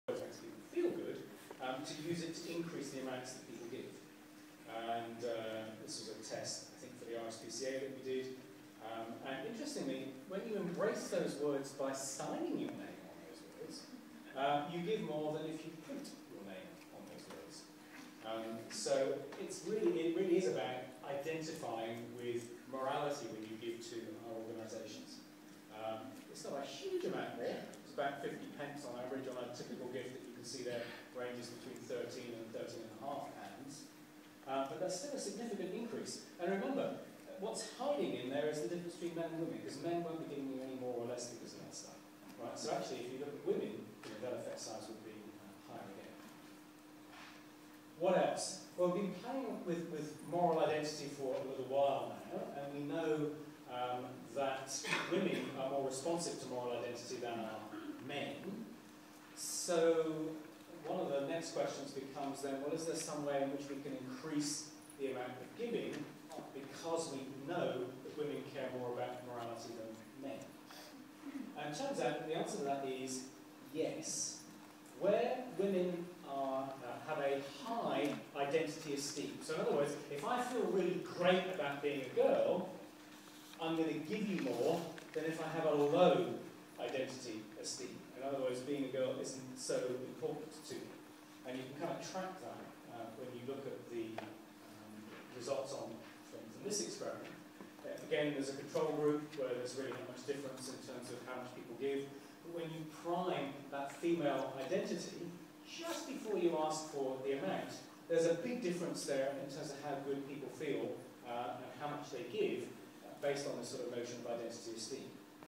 Boost a charitable gift by asking for a signature, not just a name
Professor Adrian Sargeant shares findings from research with the RSPCA in which he founded that donors that signed their name on a direct mail donation form gave more than those who were asked simply to write their names.

He attributes this to the donor's sense of moral identity. He was talking about this at the inaugural Clayton Burnett Annual Lectures in London on 5 December 2013.

"It's not a huge amount there. It's about 50p on average on a typical gift, but that is still a significant increase".

Given that women are more responsive to moral identity than men, is there some way in which fundraisers can increase the amount of giving because we know women care more about morality than men? Yes, argues Sargeant. Where women have a high identity esteem.

In the research he found that, if you prime that female identity just before you ask for the amount, there's a big difference in how much they give based on the notion of identity esteem.

So, his tips are:

* use an awareness of moral identity in your fundraising. It should boost income from women donors and not harm donations from men.

* leave it until just before you ask for the amount to offer the moral or gender identity boost

* do so in the thank you too

* remember that there are many other identities...

* don't get hung up on moral identity - think 'people' not 'motives'